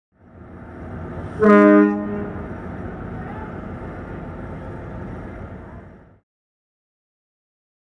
Powell River Queen's Whistle (2016)
The Whistle of the Powell River Queen sounded for sailing cut-off at Quathiaski Cove.
Recorded on March 31st, 2016 © CS16.